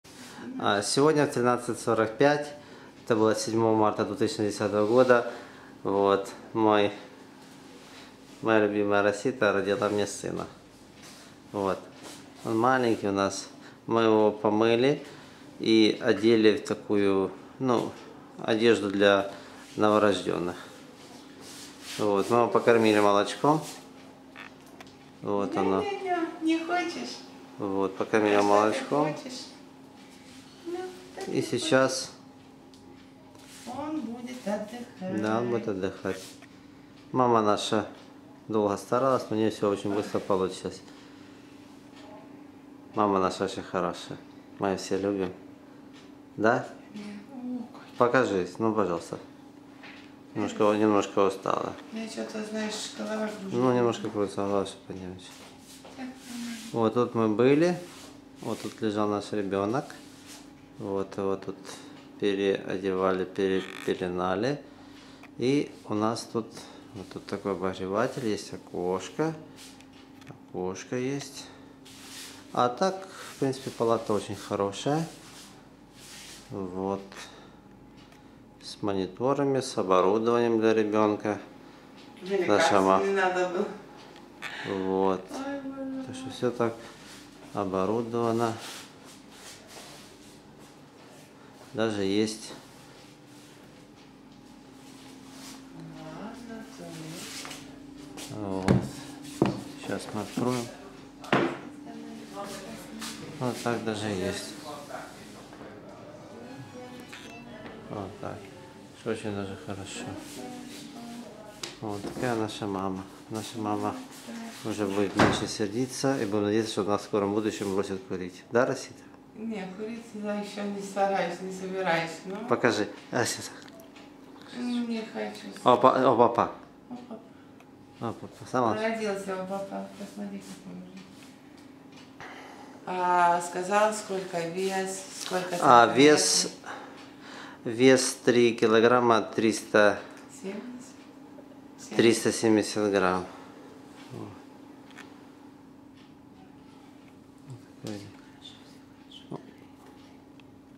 Родильная палата в Лондоне, Англия.
Как реально выглядит родильная палата в родильном отделении одного из Лондонских госпиталей в Англии.